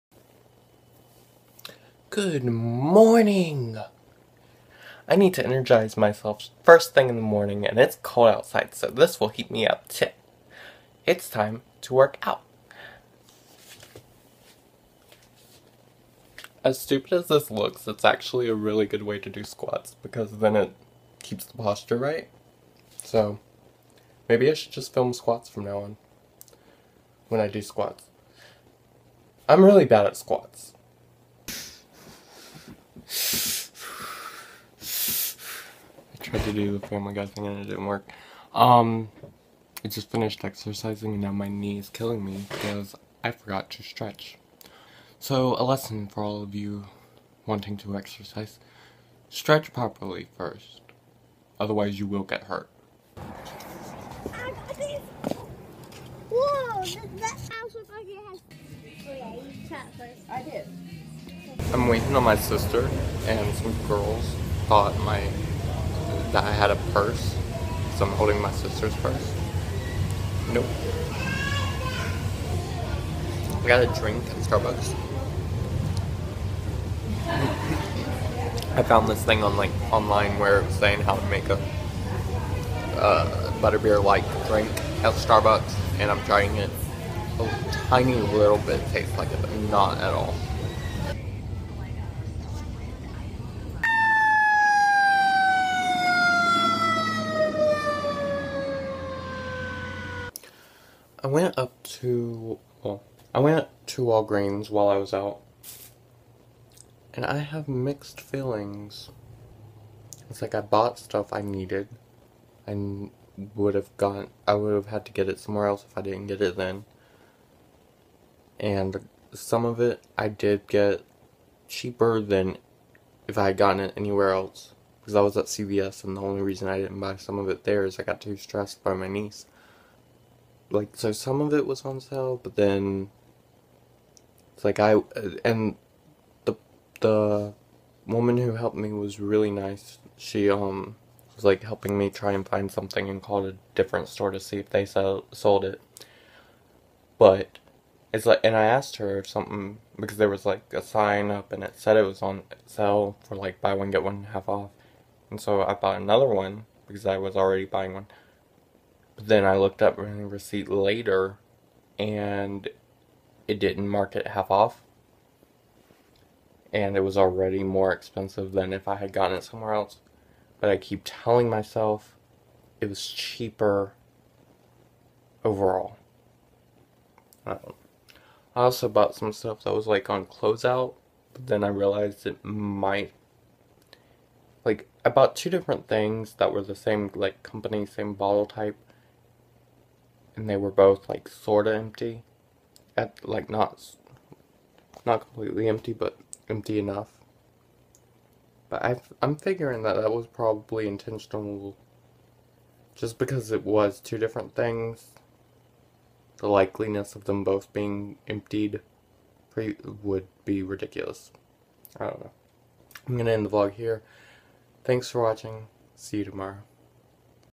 I Don't Have a Man Purse - Video Every Day January
Today, January 5th 2014, I try a Starbucks Butterbeer clone, some girls think I have a purse, and I get jipped at Walgreens.